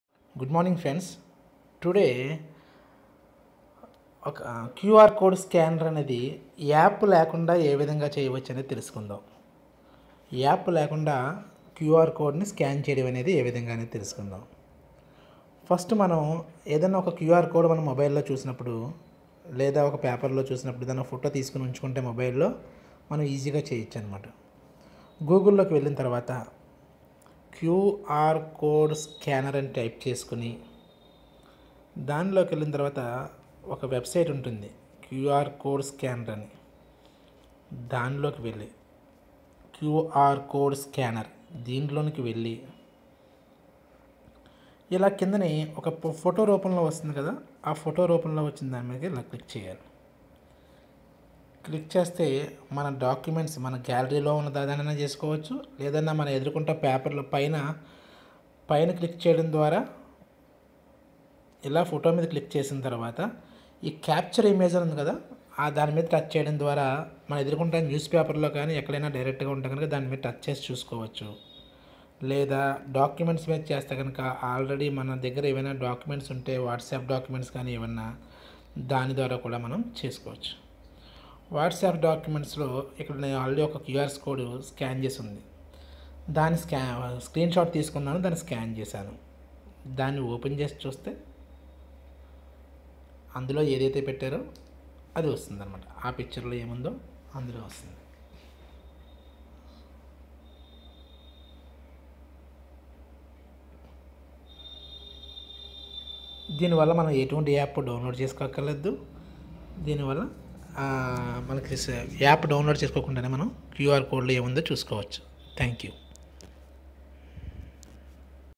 How to decode QR code
Without app we can decode any QR code... Description in telugu